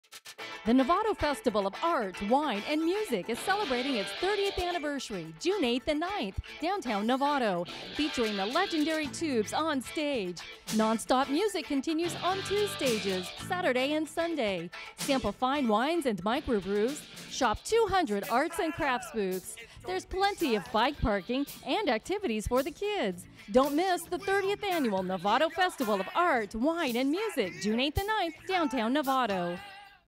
Novato Festival of Art, Wine and Music
Novato's 30th Annual Festival of Art, Wine and Music takes place on Saturday, June 8th (10 a.m. - 7 p.m.) and Sunday June 9th (10 a.m.-6 p.m.) featuring the legendary Tubes on the Main stage, Saturday, 6 p.m.  Plenty of free bike parking and activities for the whole family. From Highway 101, take the DeLong Avenue exit.  Free admission.